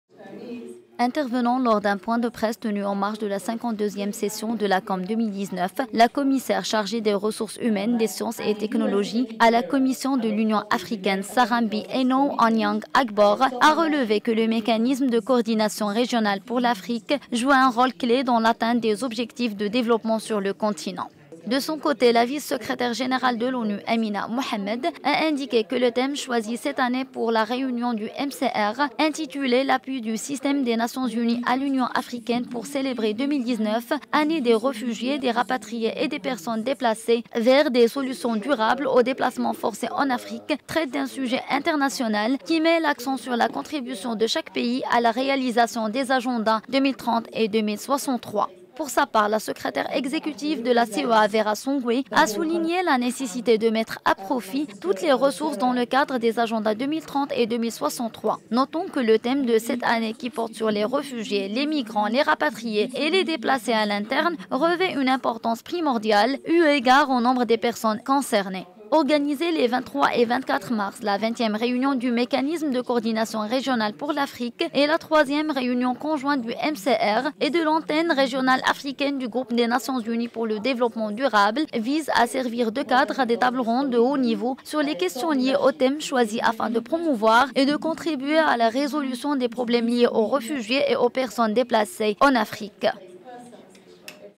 MCR, instrument clé pour atteindre les objectifs de développement en Afrique (commission de l'UA)
Lors d'un point de presse, tenu en marge de la 52è session du COM2019, la Commissaire chargée des ressources humaines, des sciences et technologies à la commission de l'Union africaine (UA), Sarah Mbi Enow Anyang Agbor, a indiqué à la ville ocre, que le mécanisme de coordination régionale pour l'Afrique (MCR-Afrique) joue un rôle clé